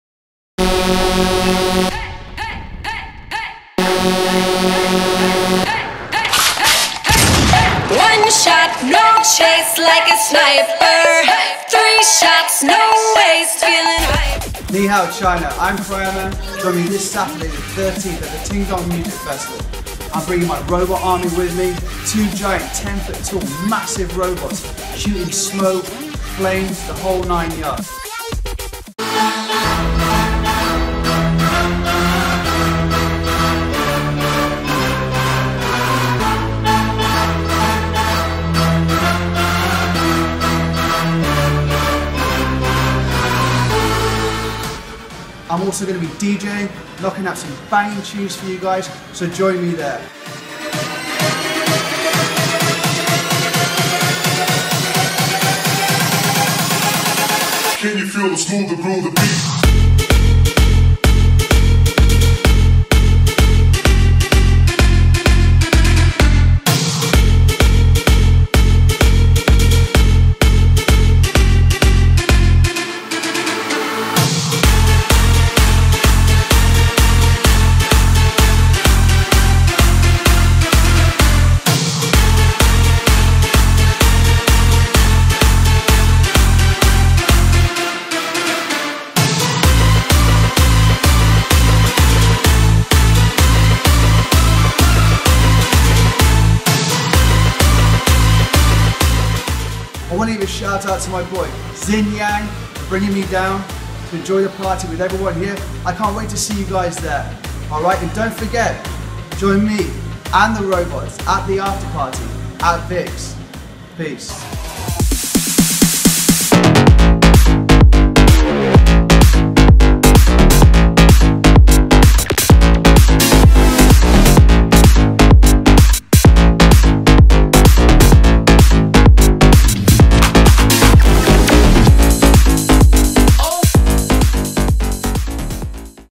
KRYOMAN - live! China debut at Workers' Stadium | Beijing
Catch both the legendary robot - KRYOMAN, and also DJ Kryoman (live) in Beijing, China!

Saturday, September 13 at Workers' Stadium (Gongti)

Sander van Doorn, Dirty South, LMFAO, Deniz Koyu, Jason Dewey, Nicole Chen (NC), Jason Taylor and many others...